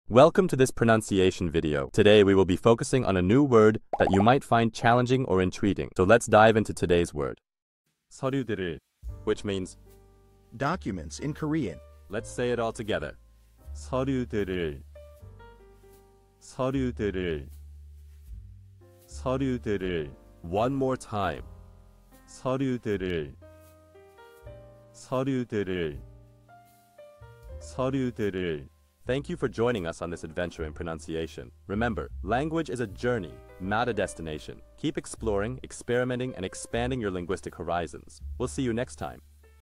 How to pronounce seoryudeureul | 서류들을 (Documents in Korean)
Master the Pronunciation of 'seoryudeureul | 서류들을' - which means : Documents in Korean 📝 with @PronunciationKorean 🎓🗣️ - [Your Guide to Korean]

Learn to pronounce 'seoryudeureul' perfectly in Korean with our easy and engaging tutorial by @PronunciationKorean! 📖📽️ In this comprehensive video, we provide you with native speaker examples 🗣️👂 and phonetic breakdowns 🎵 to ensure you get the pronunciation just right.